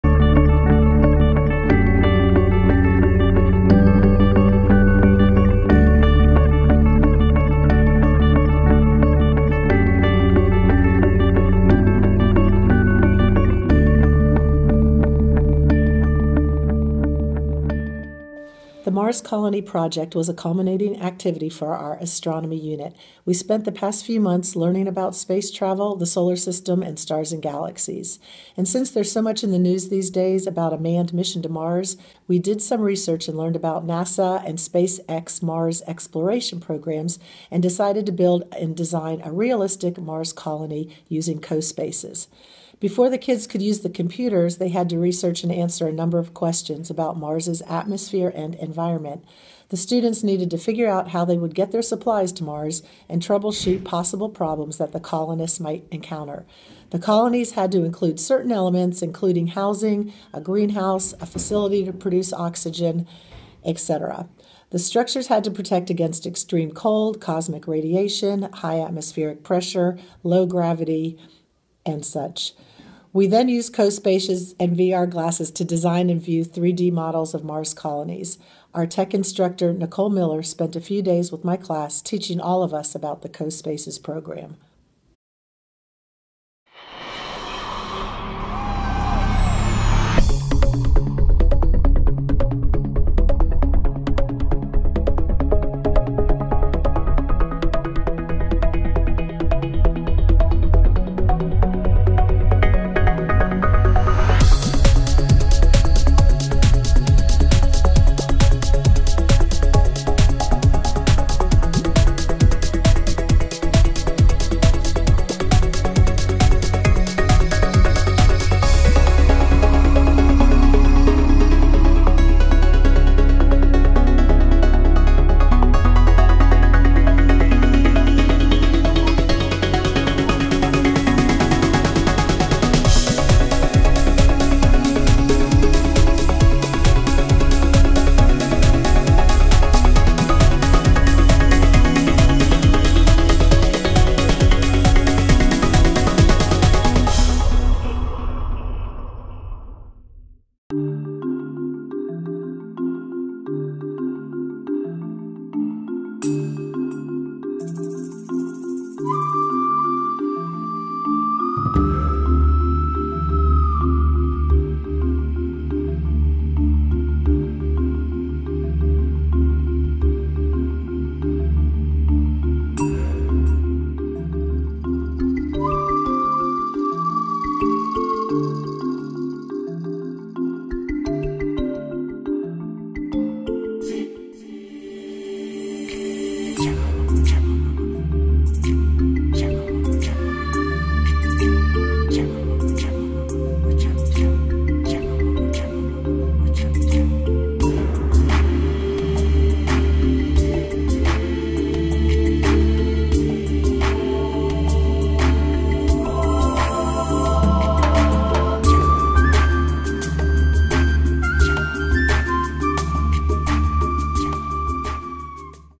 VR Mars Colonies 2022
In seventh grade earth science we used co-spaces web technology and Oculus Quest Virtual Reality (VR) goggles to design and explore a realistic Mars colony.